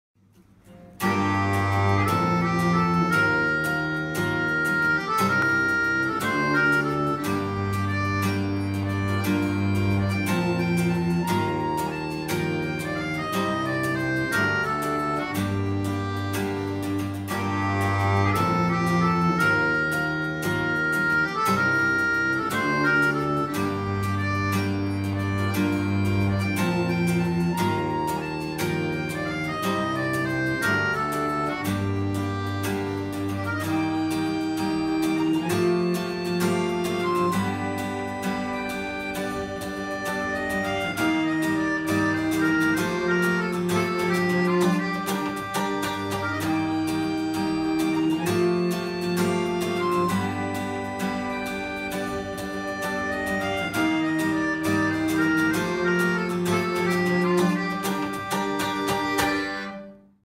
Melodica, Cello, & Guitar Jam
We're back with another video! This time, a guitarist and a cellist join me in playing a tune of mine. Hope you enjoy the music!

Cello: Rhea Khairallah
Guitar: Joy Basta
Melodica: Marc Machaalani
Filming: Romi Machaalani